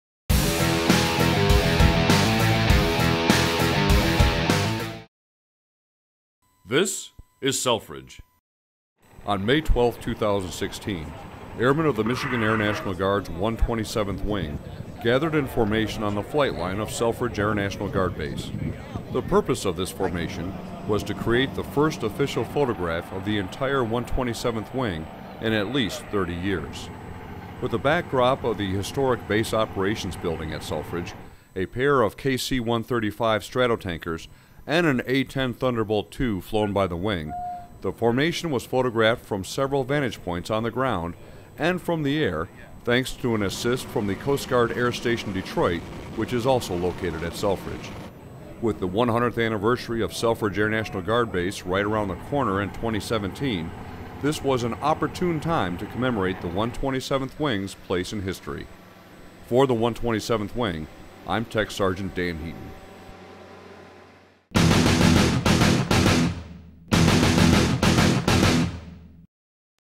127th Wing Group Photo -- How Its Made
The story behind the 127th Wing's command portrait on May 12, 2016.